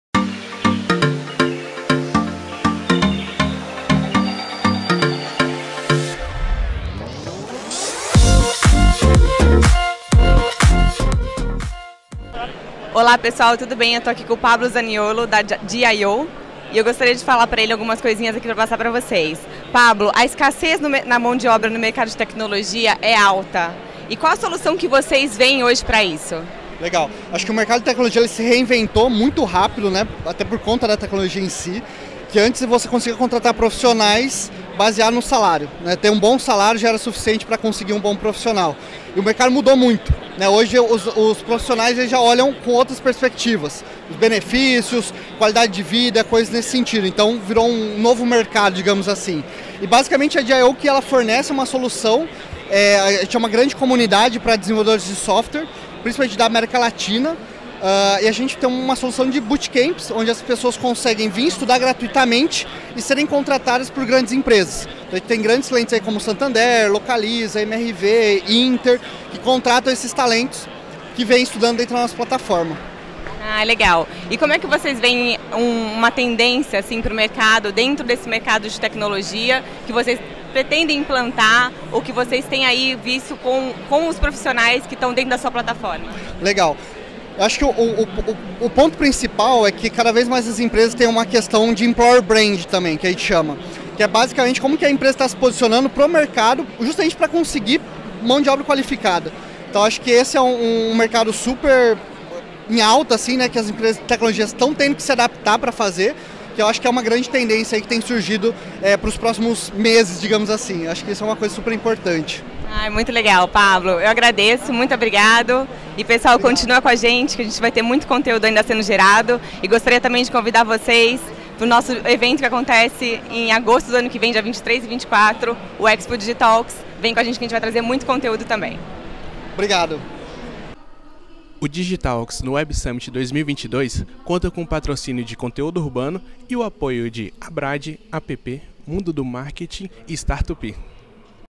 Startup brasileira conecta empresas a programadores
Pablo Zaniolo, COO da DIO, conta sobre o desafio de empresas encontrarem talentos na área de tecnologia.

Acompanhe a principal cobertura da maior conferência de tecnologia da Europa